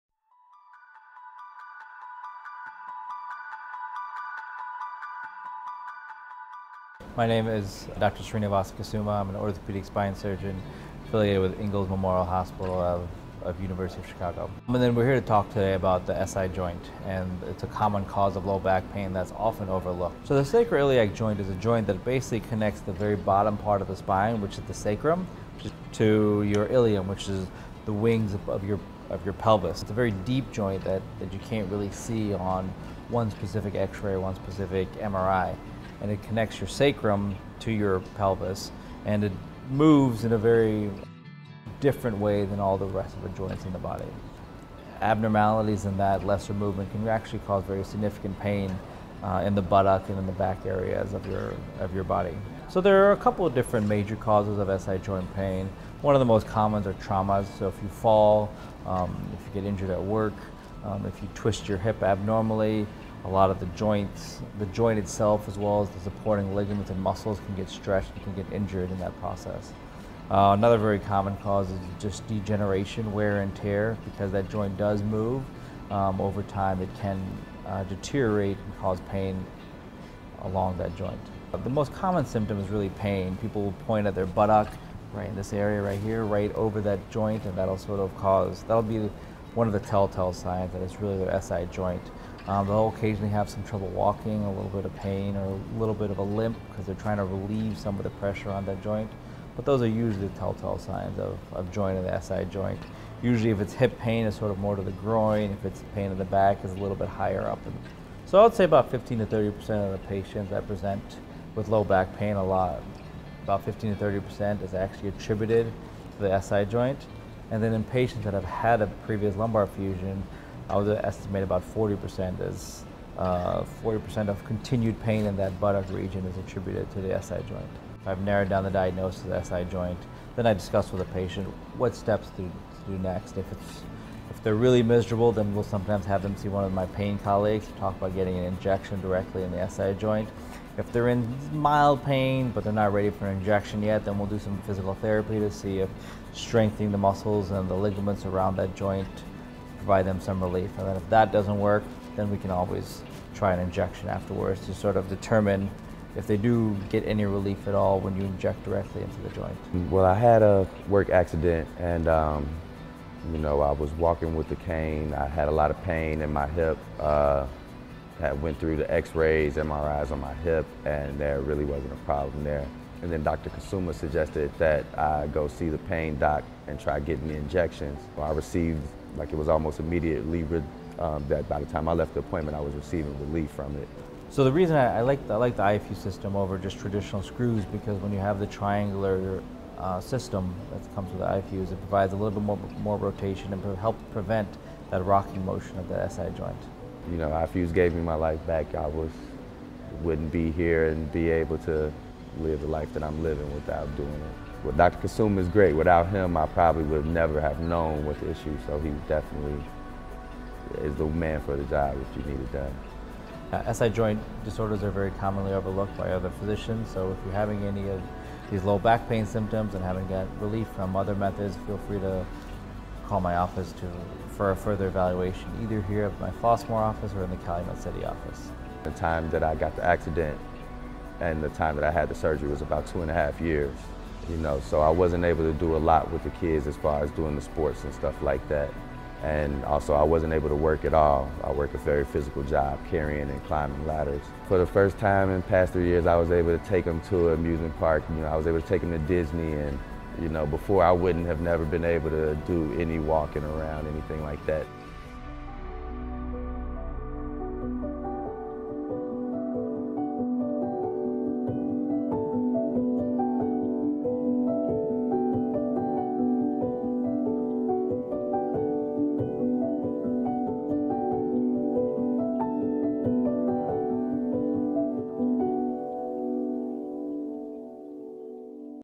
So You're Diagnosed with SI Joint Dysfunction, What's Next? - Dr. Kusuma
Srinivasu Kusuma, MD at UChicago Medicine Ingalls Memorial Hospital and his patient discuss SI joint dysfunction and the next steps after diagnosis.